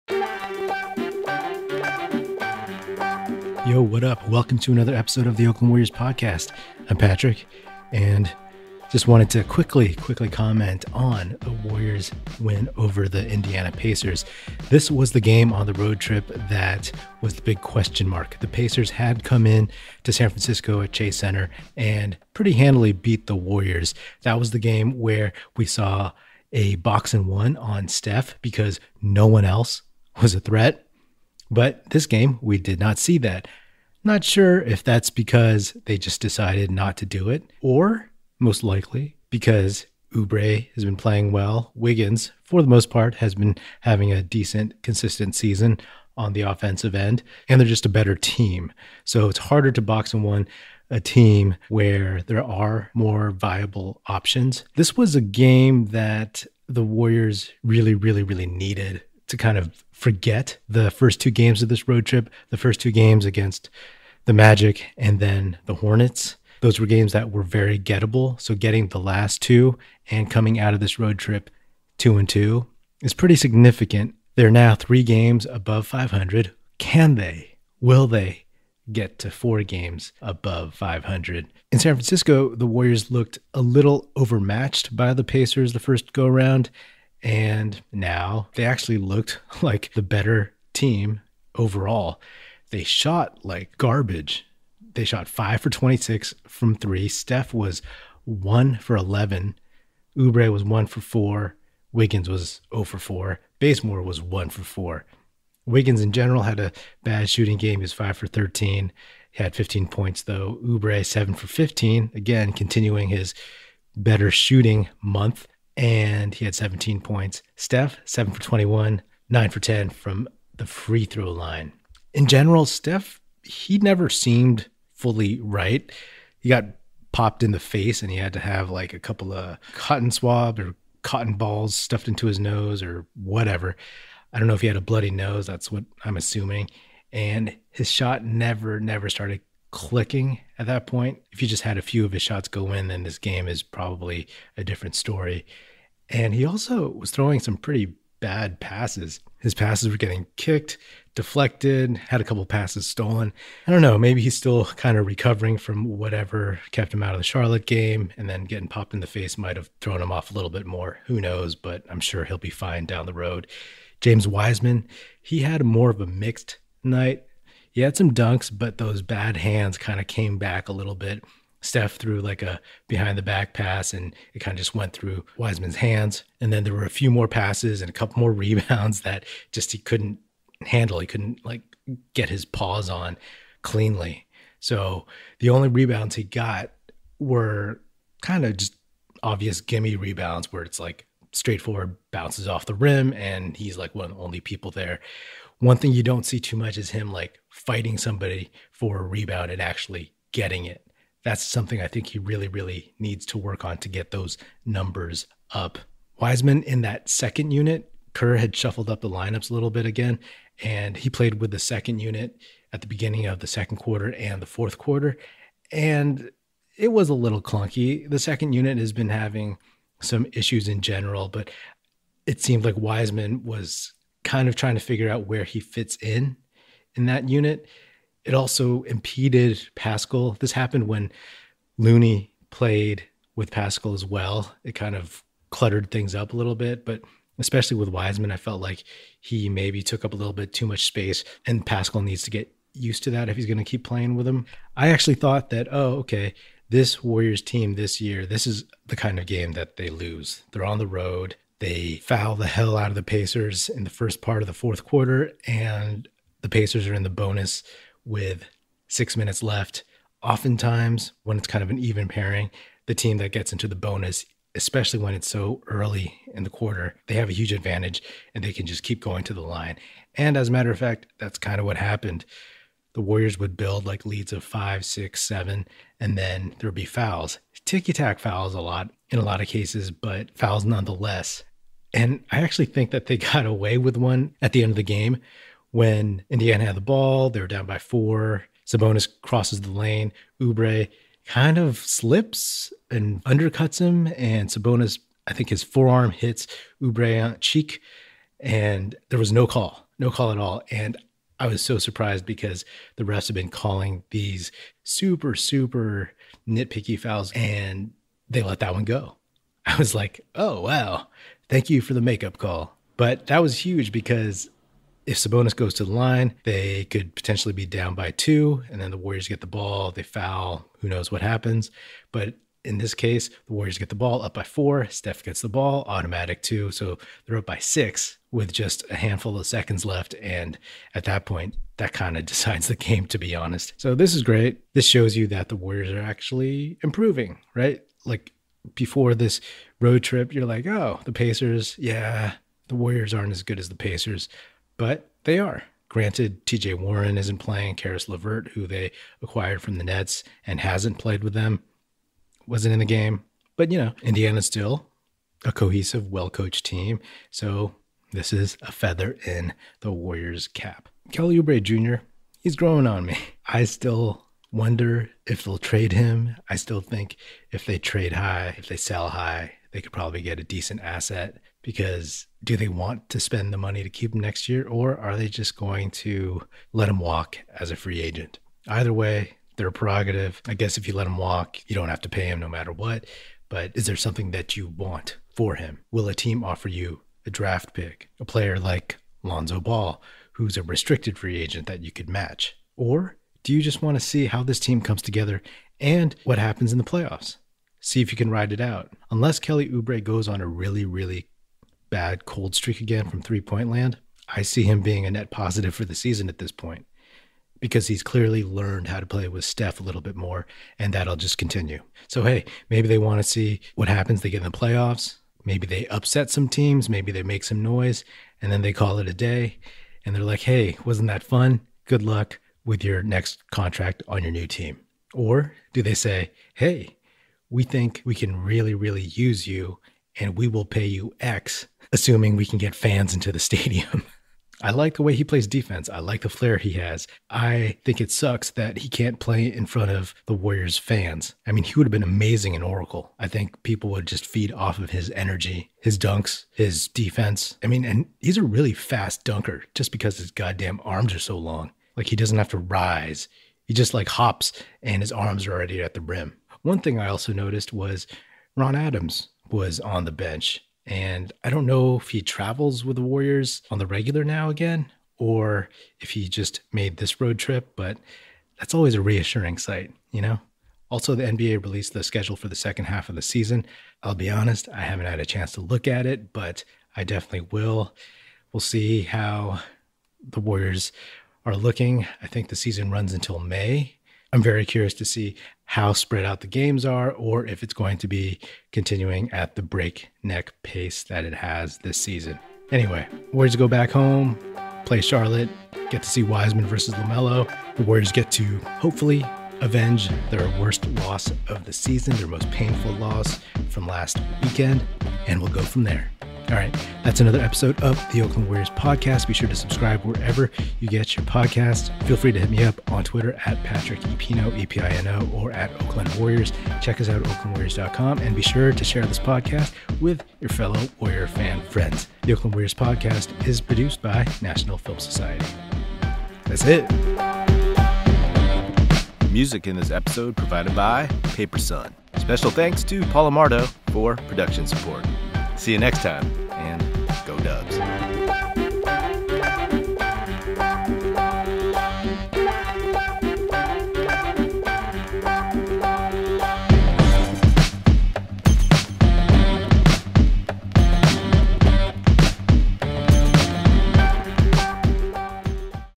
Oubre Easier or Harder to Trade? Steph’s Off Shooting Night, Wiseman’s Mixed Bag, Beat IND | Ep. 58
Original airdate: 2/25/2021 – Patrick drops his observations on the Warriors win over the Indiana Pacers. Even without facing a box-and-1 from Indiana, Stephen Curry had an off night shooting but the W’s did enough to come back home from the road trip 2-2. Steve Kerr keeps trying new lineups and rotations with James Wiseman in the second unit. Plus, Kelly Oubre Jr. continues to play well. But does that make it harder to part with him or easier to trade him?

HOST:
Patrick Epino

CONNECT WITH THE PODCAST!